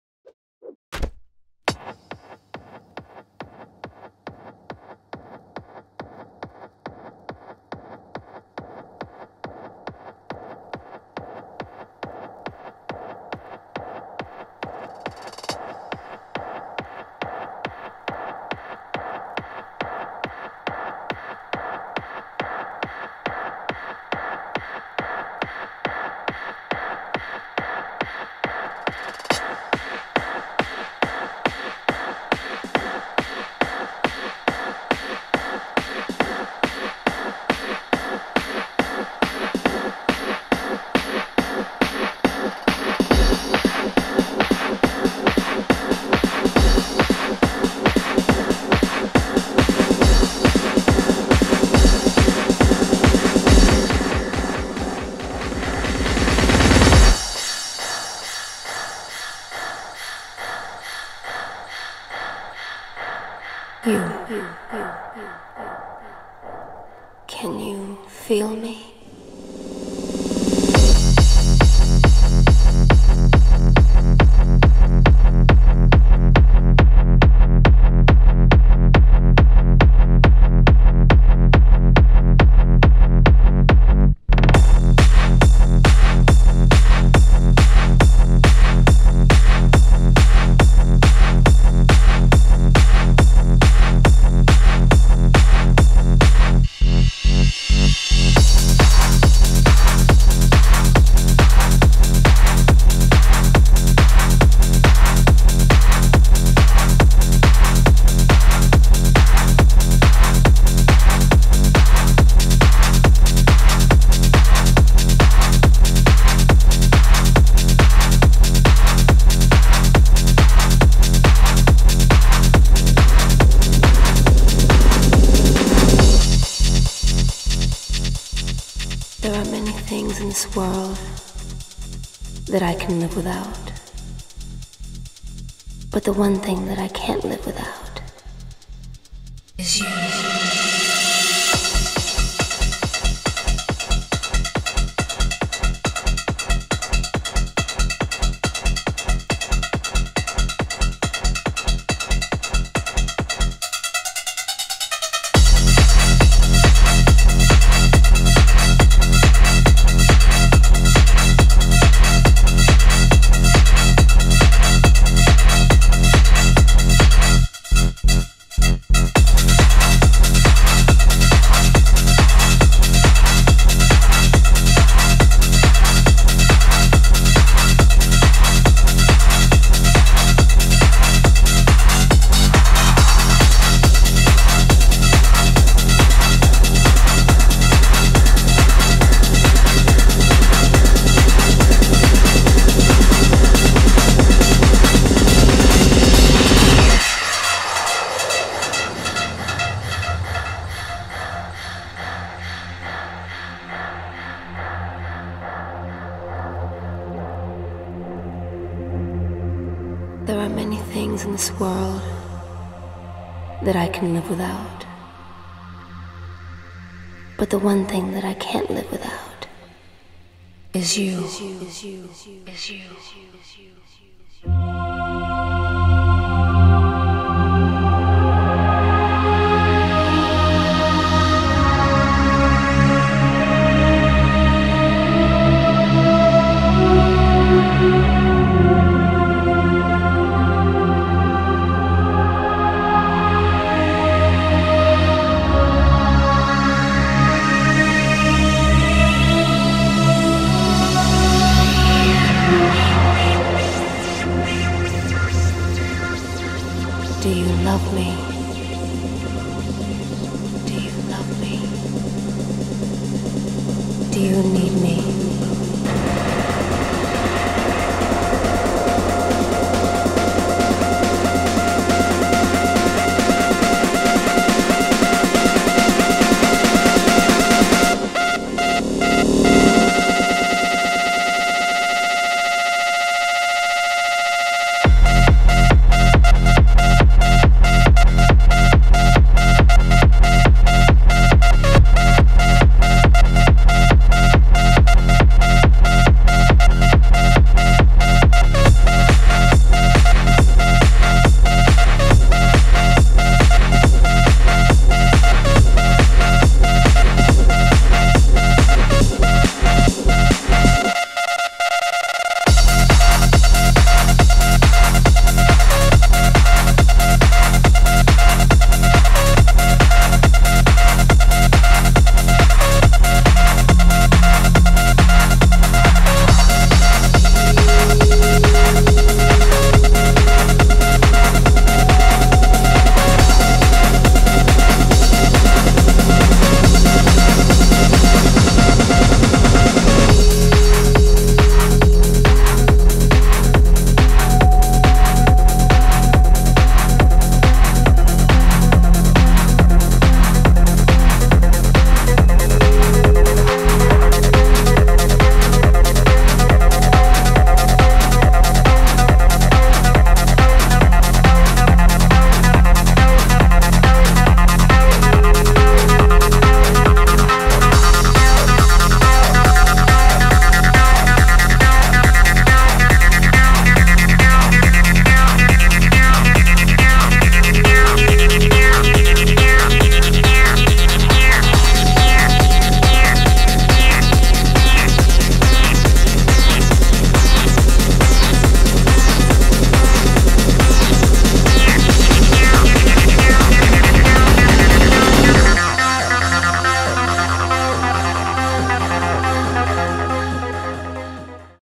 Can You Feel Me
A transmission to help clear our stuck emotions in the sacral chakra. Also I work with clearing the water at Cannon Hill Park.